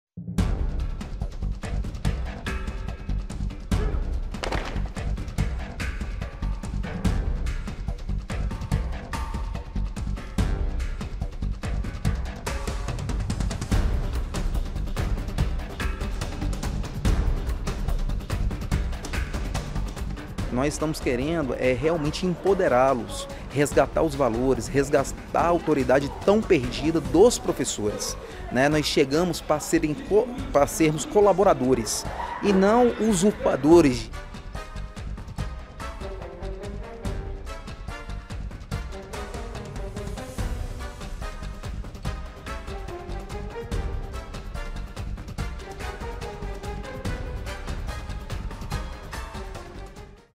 Brasilia ya tiene colegios públicos con gestión militar
El modelo de colegio militar que promueve el presidente brasileño Jair Bolsonaro comienza su periodo de prueba en la periferia capitalina, donde casi 7.000 alumnos serán parte de un plan piloto que comenzó a aplicarse en cuatro centros educativos públicos del Distrito Federal. VERSIÓN SUBTITULADA